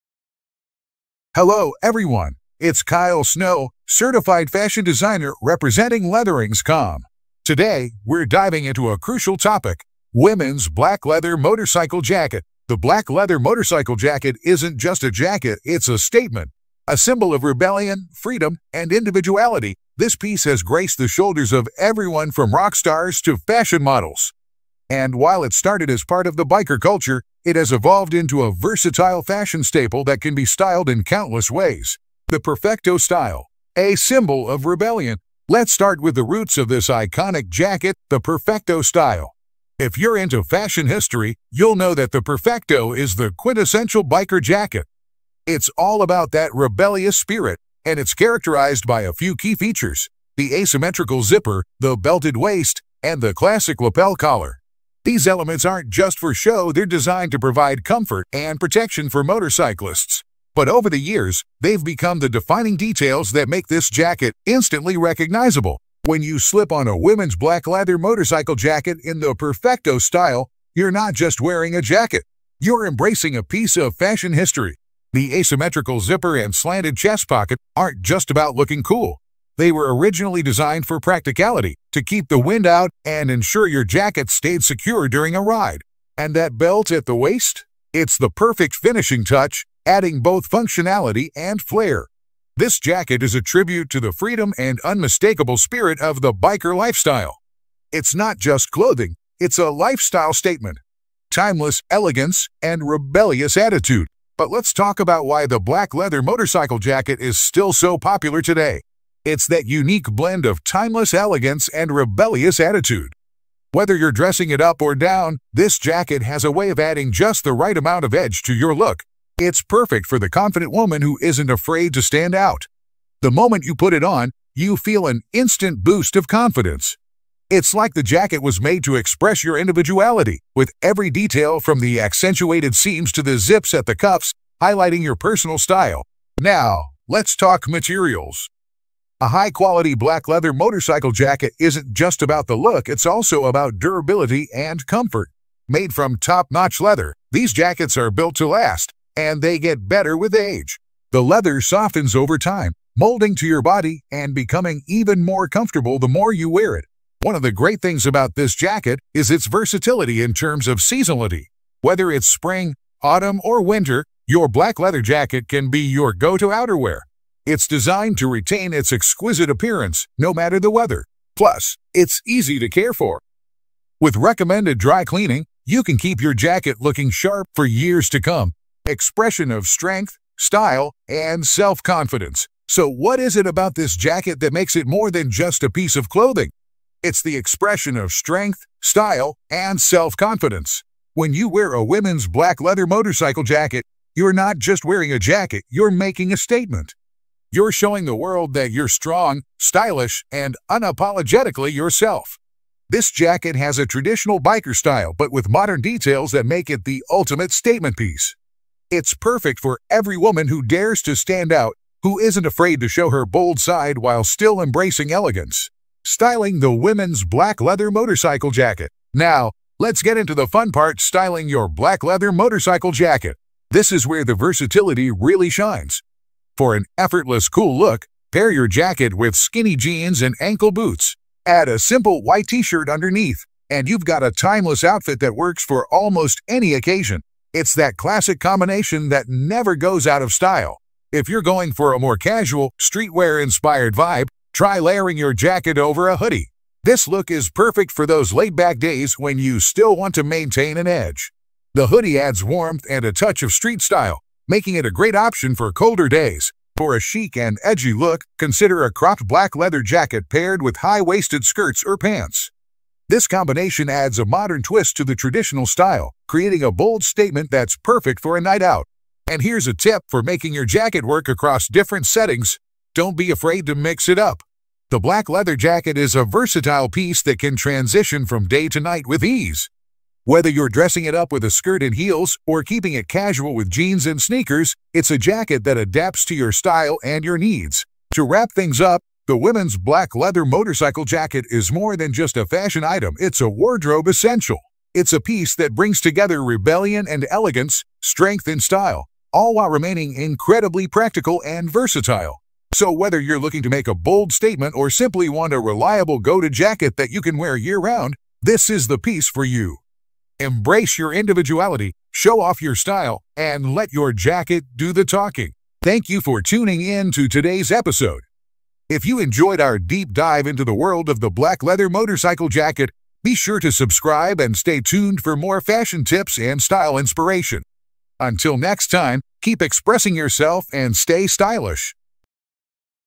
Womens Black Leather Motorcycle Jacket
Welcome to our fashion channel, where we explore the most iconic and must-have pieces for your wardrobe. In today’s video, we're focusing on a true fashion staple that never goes out of style—the Women's Black Leather Motorcycle Jacket.

The black leather motorcycle jacket is more than just an outerwear piece; it's a symbol of rebellion, freedom, and individuality. Originally inspired by the classic "Perfecto" biker style, this jacket has evolved into a versatile garment that can be dressed up or down, making it an essential addition to any wardrobe.

What You’ll Discover in This Video:

History and Iconic Design: We'll delve into the origins of the black leather motorcycle jacket, exploring its roots in biker culture and how it became a global fashion phenomenon. Learn about the "Perfecto" style, known for its asymmetrical zipper, belted waist, and classic lapel collar.

Style and Versatility: We’ll show you how this jacket can be the perfect finishing touch for a variety of outfits. Whether you’re pairing it with skinny jeans and boots for an effortless cool look, layering it over a hoodie for casual streetwear vibes, or combining it with a chic skirt for an edgy night-out look—this jacket does it all.

Quality and Care Tips: Discover why high-quality leather, particularly lambskin, is the preferred material for these jackets. We’ll provide tips on how to care for your jacket to ensure it remains a wardrobe staple for years to come, from regular conditioning to proper storage.

Why It’s a Must-Have: We’ll discuss why the Women’s Black Leather Motorcycle Jacket is more than just a trend. It’s an investment in a timeless piece that exudes confidence, strength, and style, perfect for any season and occasion.

This video is perfect for fashion enthusiasts who want to learn more about the history, versatility, and styling tips for the Women’s Black Leather Motorcycle Jacket. Whether you’re a longtime fan of this iconic jacket or considering adding one to your collection, you won’t want to miss this in-depth look at what makes it so special.